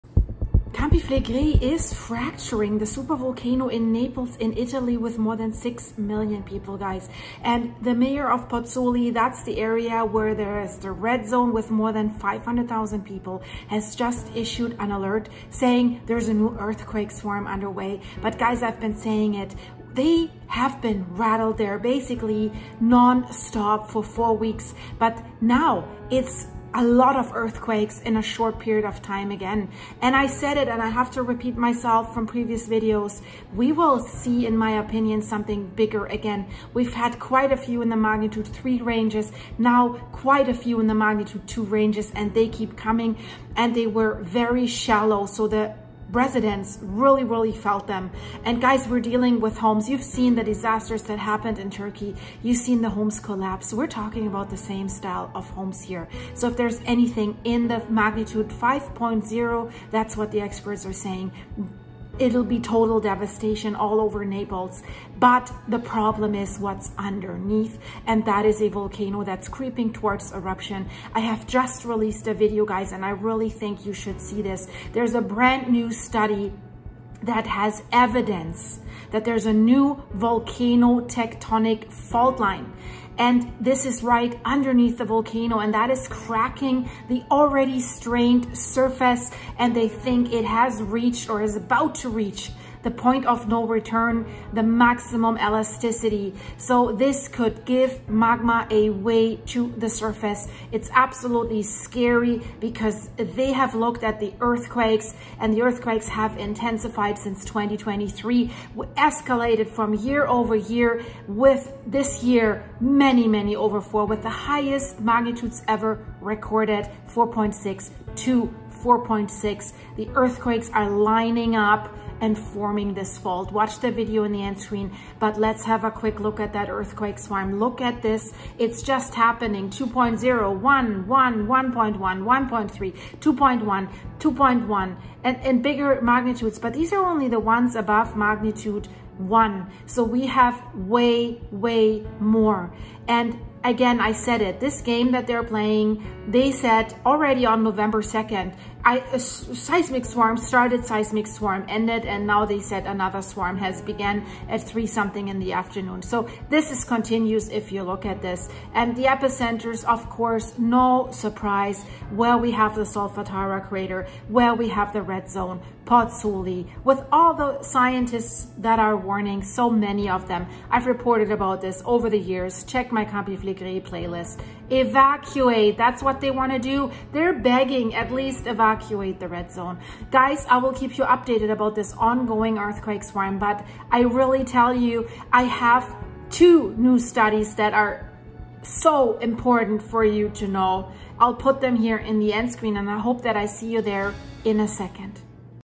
Population RATTLED by Shallow Earthquakes ! Concerns about the Crust of the Super Volcano in Naples
Over 4 Weeks now this Earthquake Swarm at the Campi Flegrei Super Volcano under Pozzuoli and Naples in Italy is lasting and residents are worn out by the constant fear. The latest NEW Study adds to that concern A LOT !

🍵  THANK YOU so much for your support it is much appreciated and helps a lot  ! ❤️

This video contains music from Shutterstock, licensed by Splice video editing app.